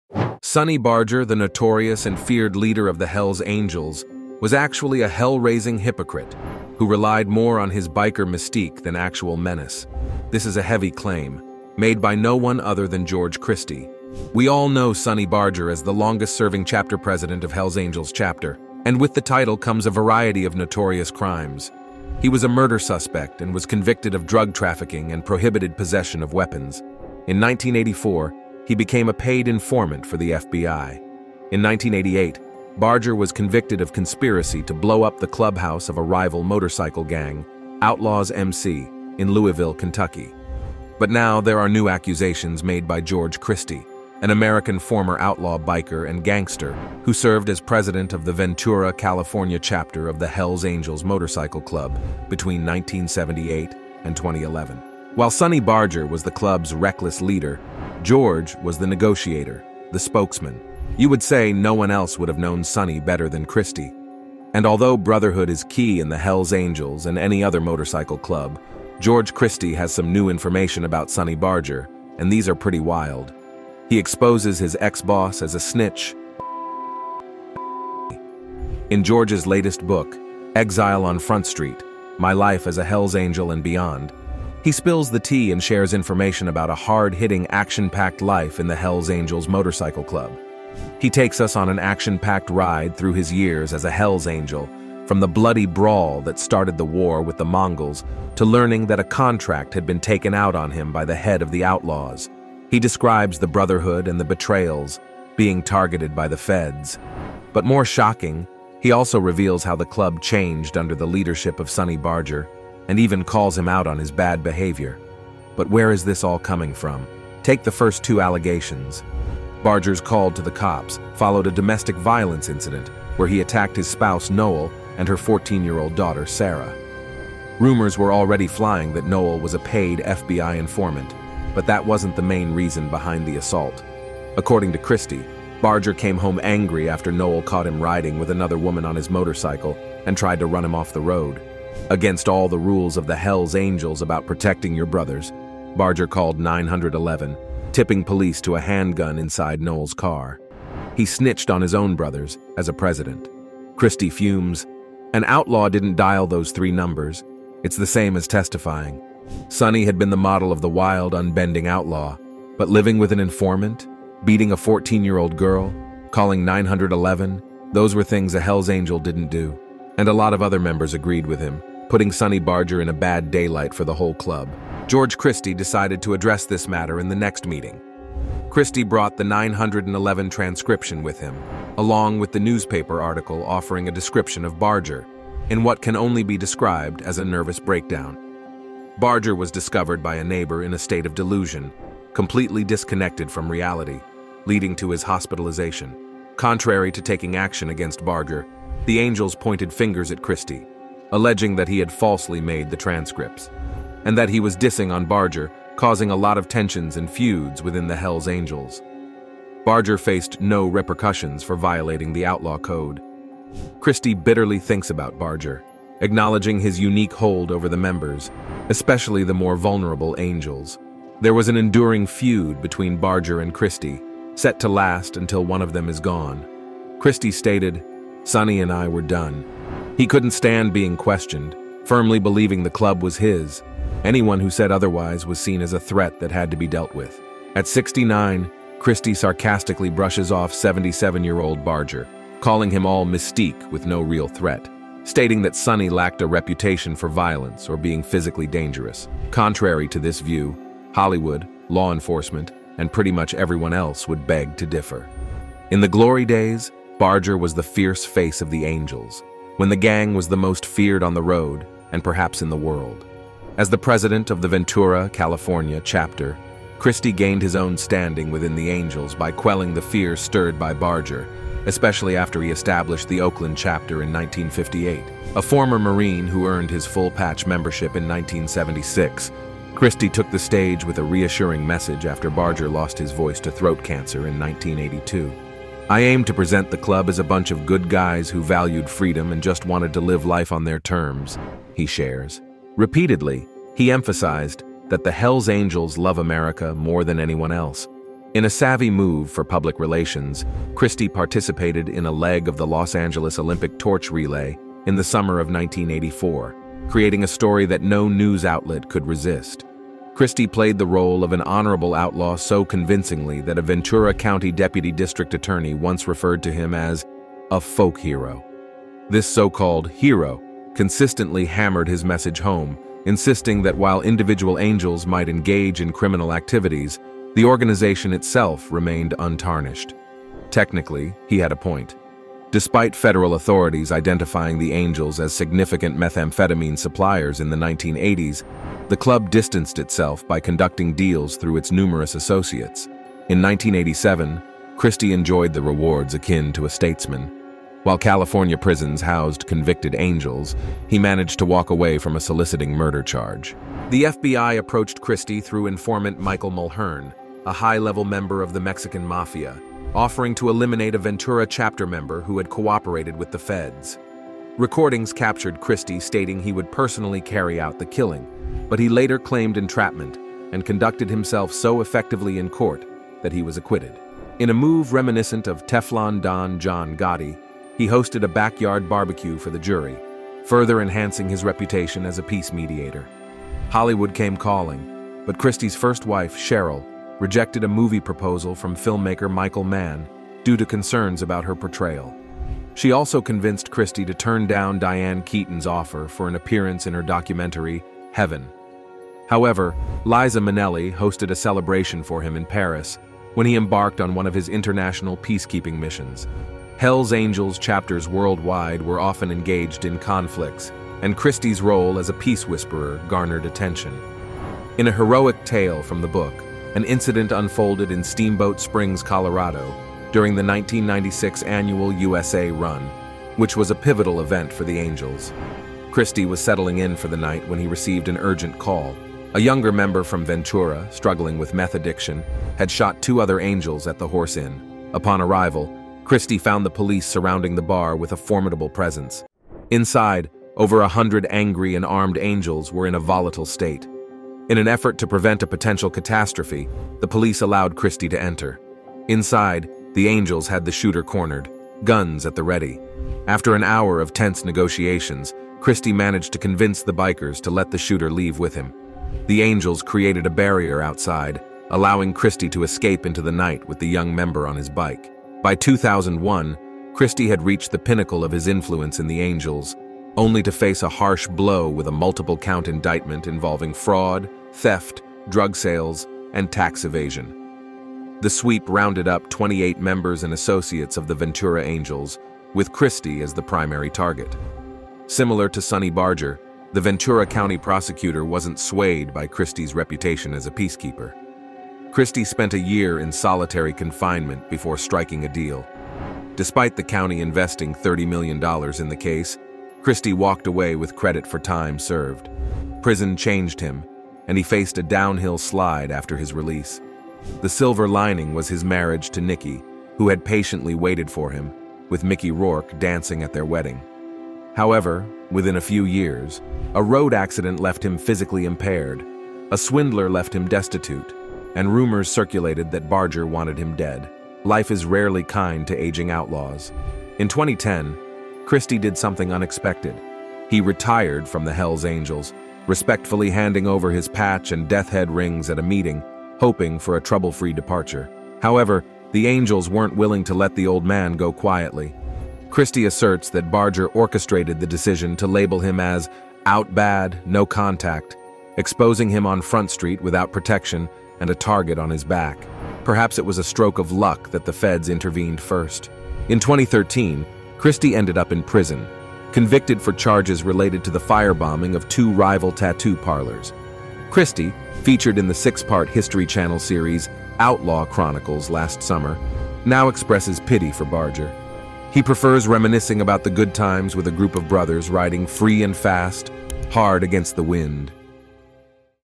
Why Sonny Barger Was Nothing But A Hell Raising Hypocrite
Why Sonny Barger  Was Actually A Hell Raising Hypocrite

“Sonny Barger the notorious and feared leader of the Hells Angels, was actually a hell-raising hypocrite, who relied more on his biker mystique than actual menace.”

This is a heavy claim, made by no one other than George Christie. We all know Sonny Barger as the longest-serving chapter president of Hells Angels Chapter. And with the title comes a variety of notorious crimes. He was a murder suspect and was convicted of drug trafficking and prohibited possession of weapons. In 1984, he became a paid informant for the FBI. In 1988, Barger was convicted of conspiracy to blow up the clubhouse of a rival motorcycle gang, Outlaws MC, in Louisville, Kentucky. But now there are new accusations made by George Christie, an American former outlaw biker and gangster who served as president of the Ventura, California chapter of the Hells Angels Motorcycle Club between 1978 and 2011. While Sonny Barger was the club’s reckless leader, George was the negotiator, the spokesman. You would say no one else would have known Sonny better than Christie. And although brotherhood is key in the Hells Angels and any other motorcycle club, George Christie has some new information about Sonny Barger and these are pretty wild. He exposes his ex-boss as a snitch, a wife-beat*er and a phony.

Buy your favorite biker clothes & accessories:

Cleaning Kit